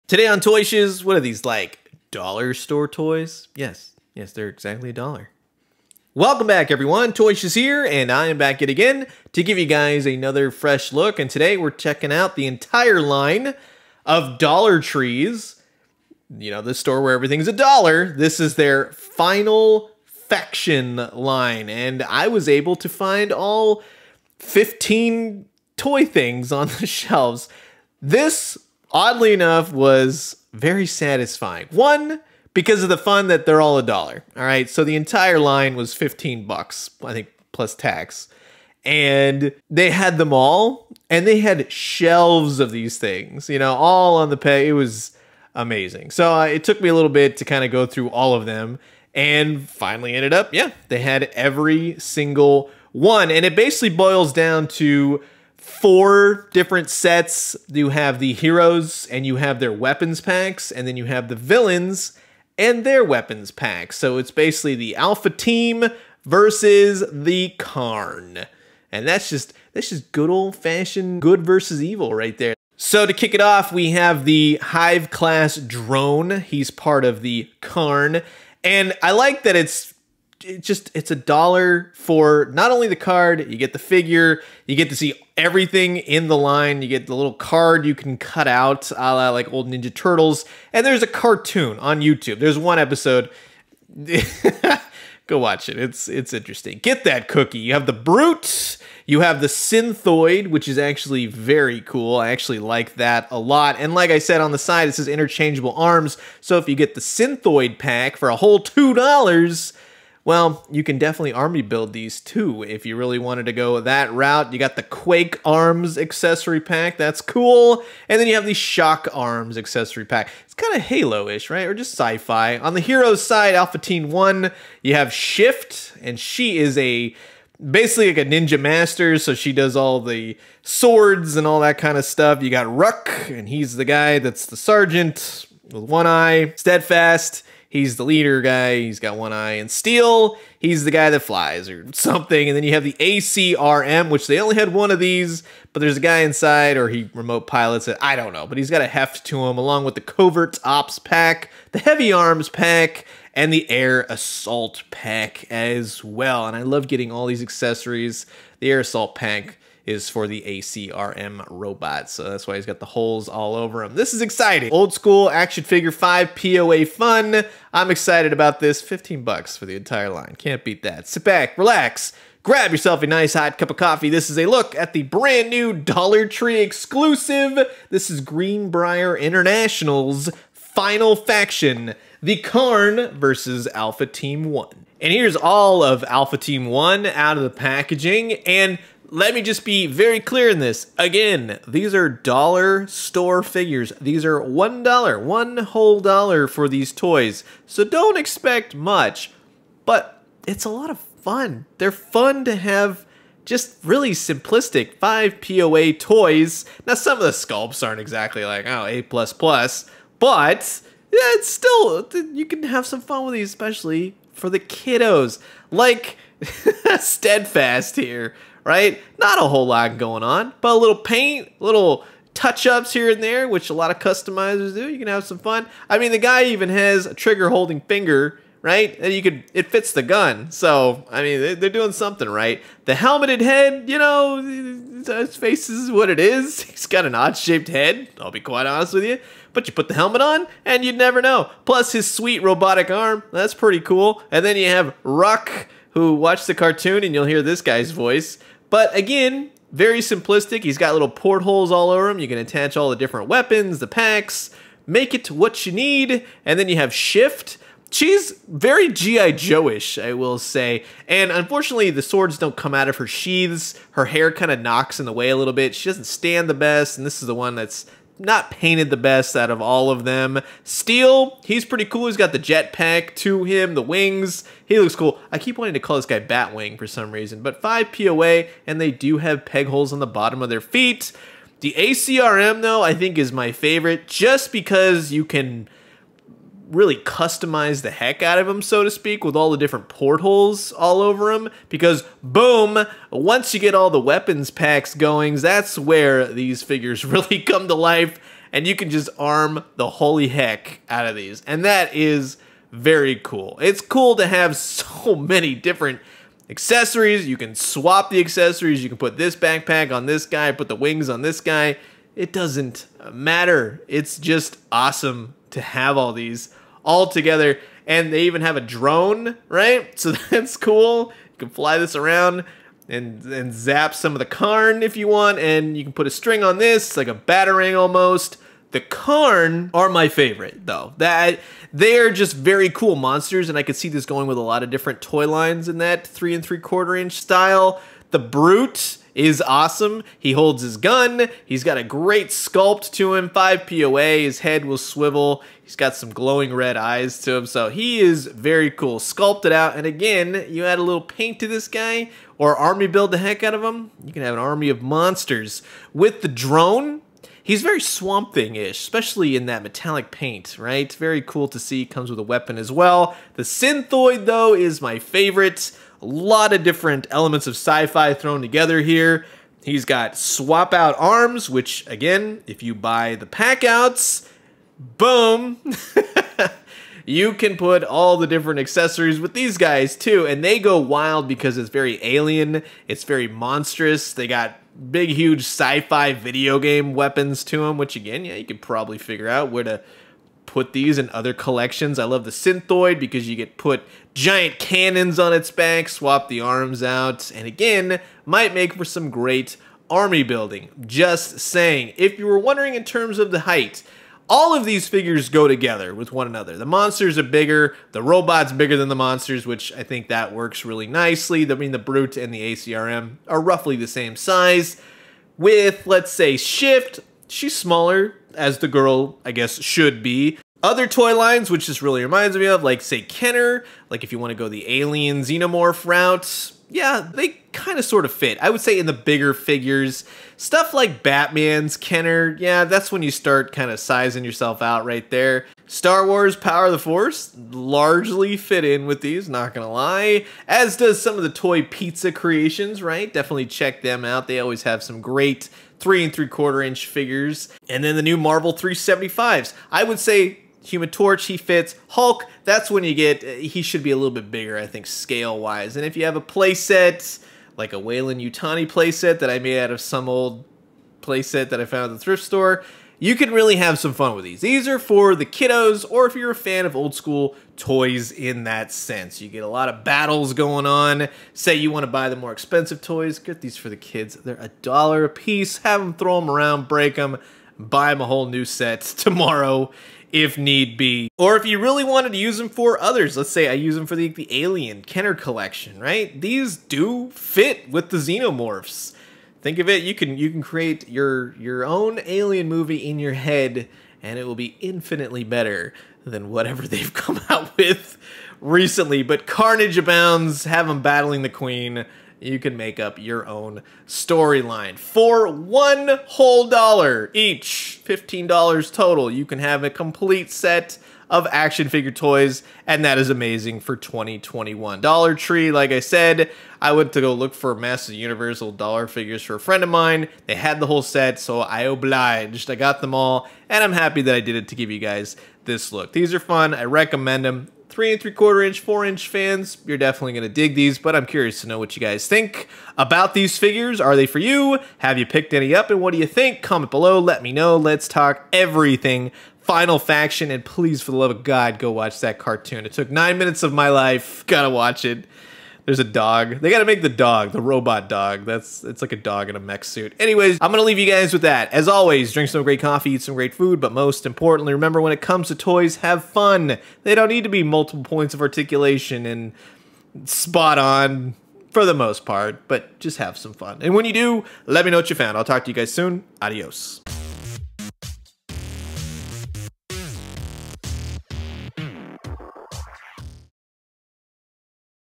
2021 The $1 Toy Line: FINAL FACTION Wave One
Everything you need to know about the $1 toy line.
FINAL FACTION that’s exclusive at DOLLAR TREE!